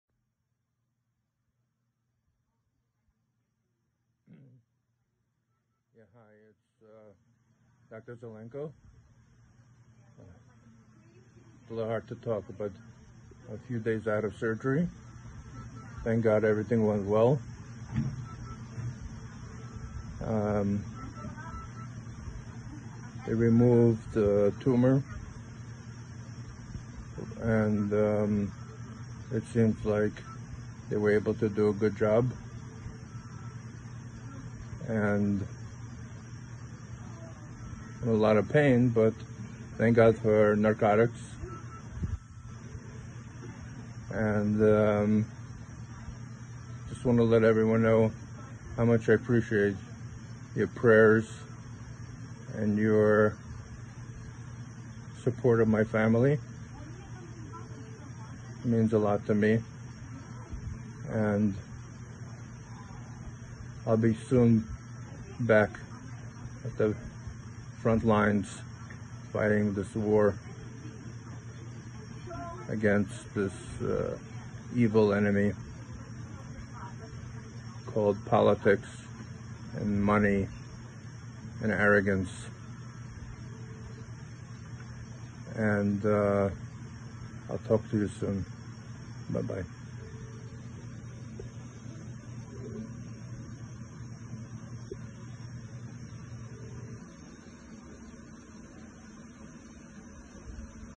Dr. Zlenko. After the heart surgery. First video.
The Internet Protocol team hopes for a quick recovery of Dr. Vladimir Zev ben Lea Zelenko. We are appealing to all good people in the world to pray for his recovery, adding in acts of goodness and kindness.